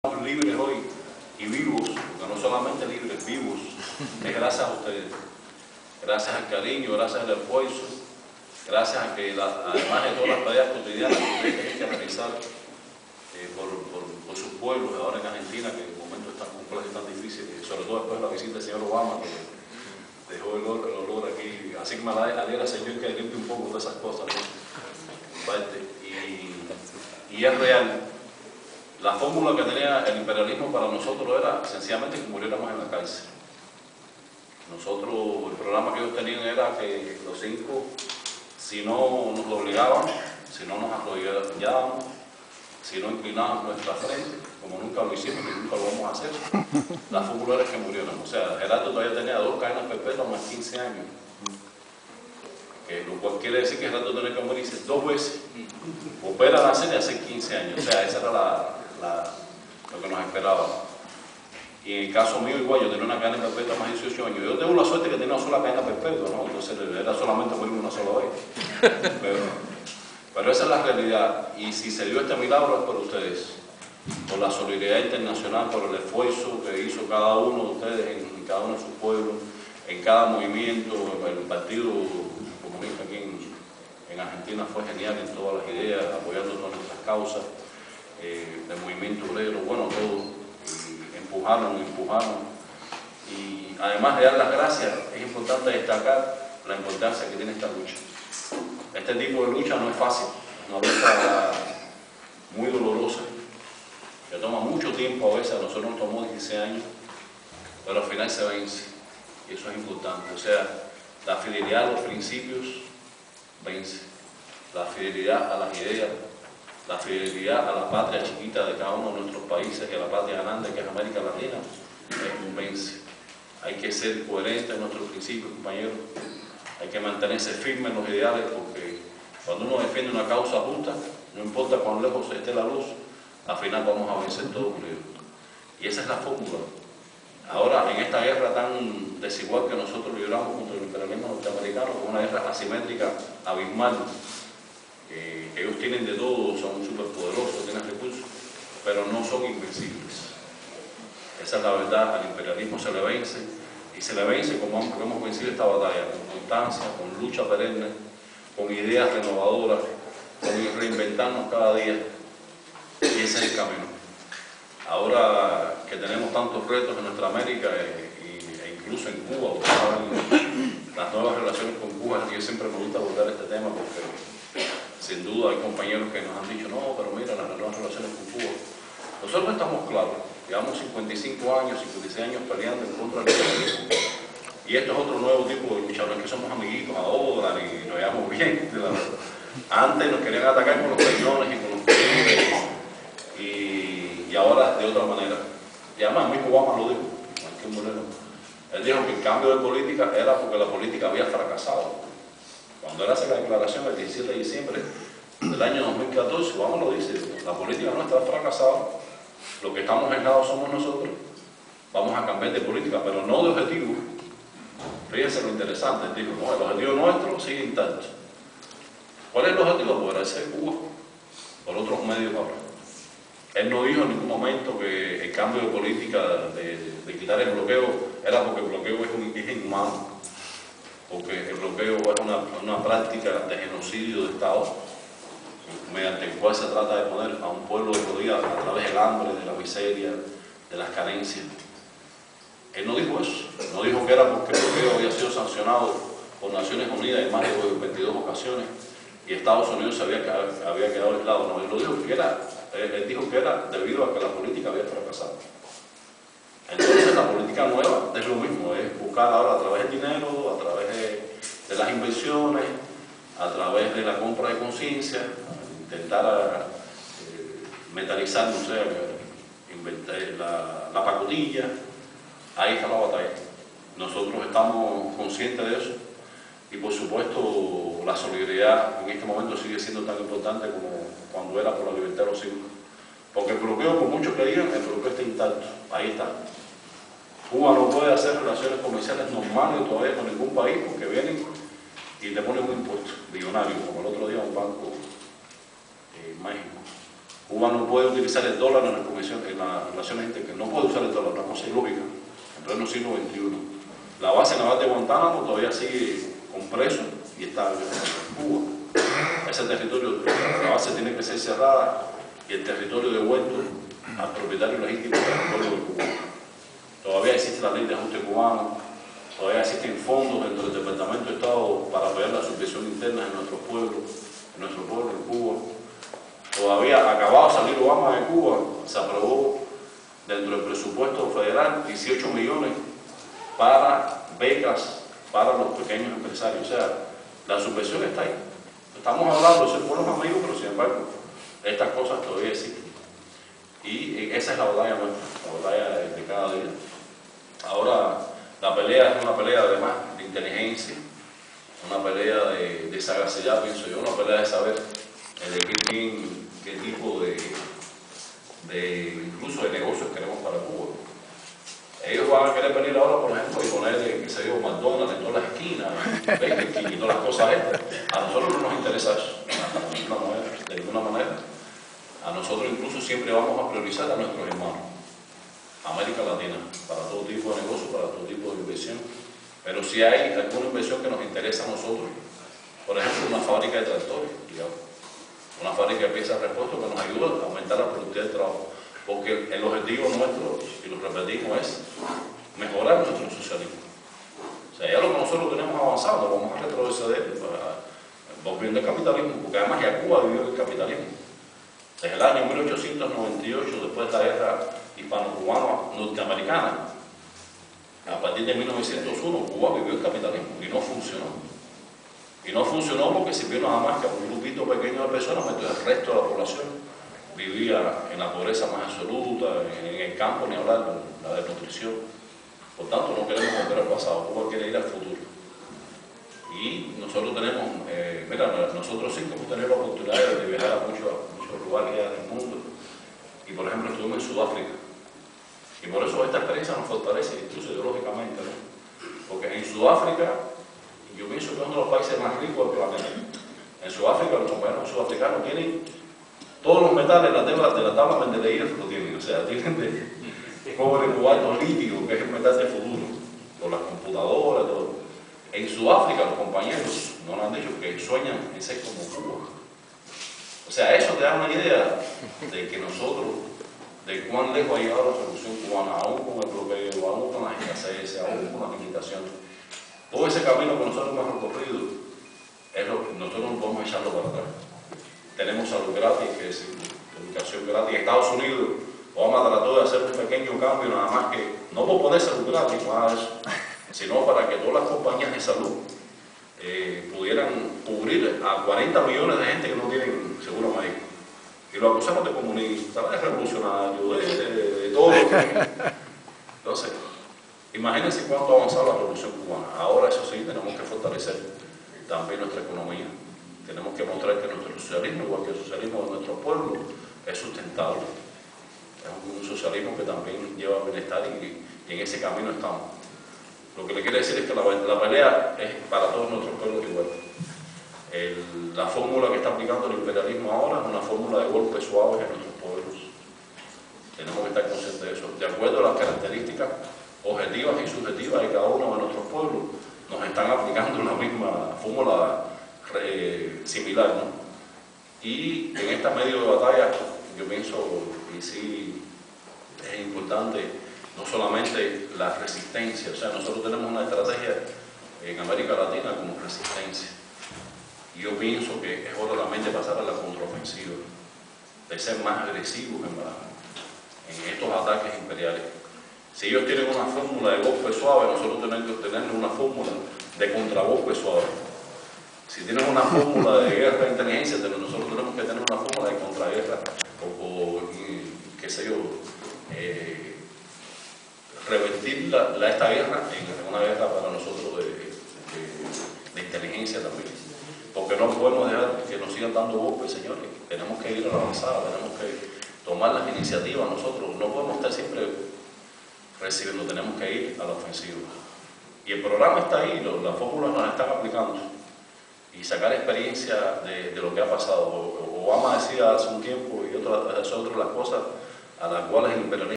Matrimonio de Héroes Ramón Labañino y Elizabeth Palmiero su esposa hablaron la CABA Argentina.
ella presa con sus dos hijos de la desaparición en Estados Unidos de su esposo.  El Héroe de la República de Cuba resistió la tortura y el encierro durante quince años, o tres lustros,